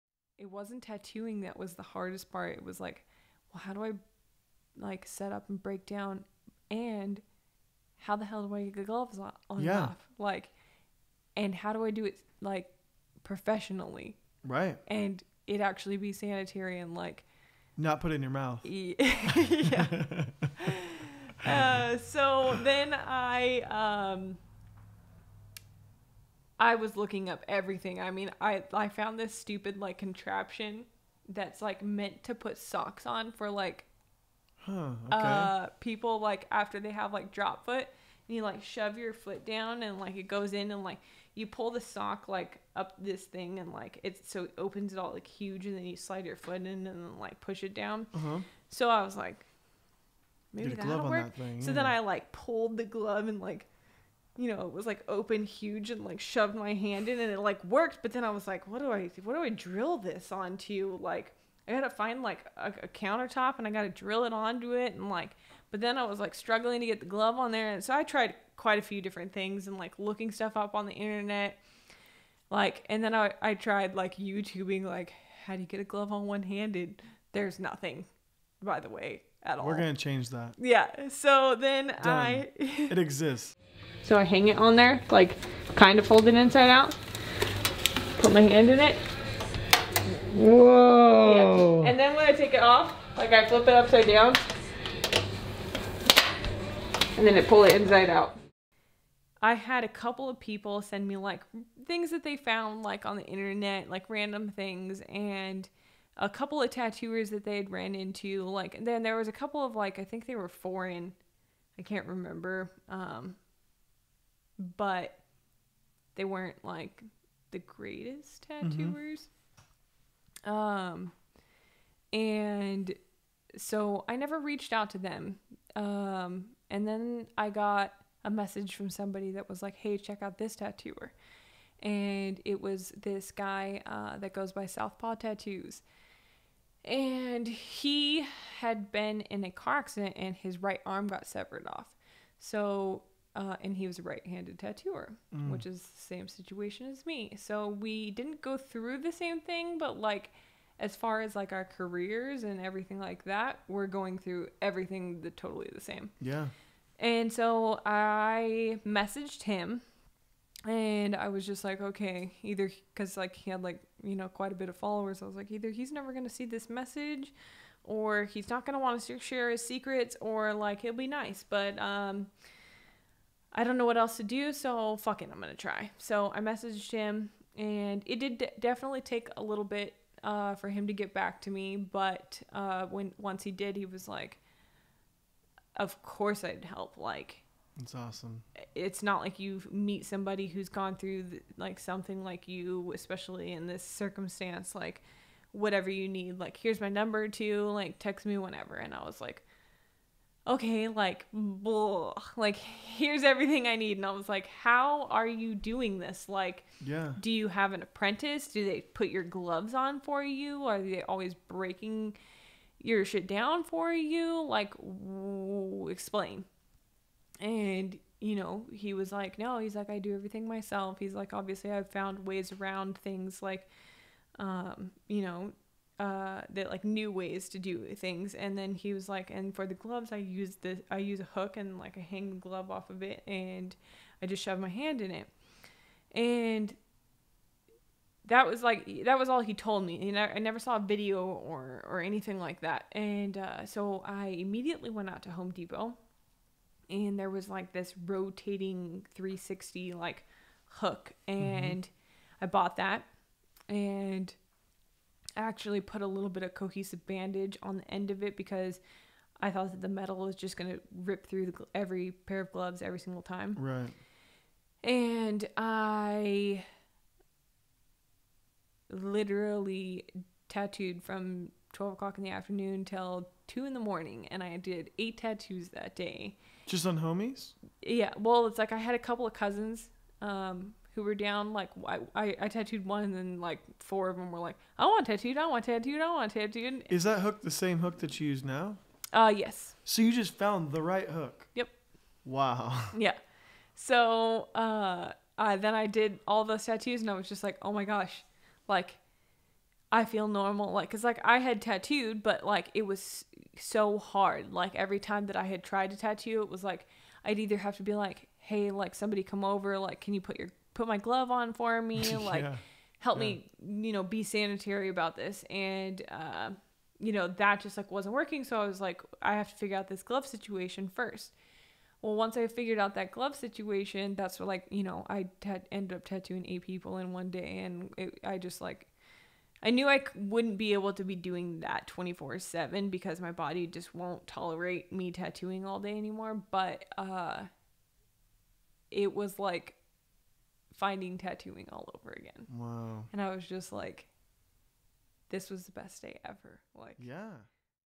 How to put on a glove one handed
This is an excerpt from Ep. 13 of Unite and Win Podcast.
Stevie Randallyn is a Tattooer and a Mother and one of the most strong willed people I've ever met. She has had re learn to tattoo with her left hand after surviving a stroke, and miraculously Stretches the skin and tattoos
all just one handed now. Only two years since the stroke she is on the road doing guest spots, tattooing between Arizona, Las Vegas, and California.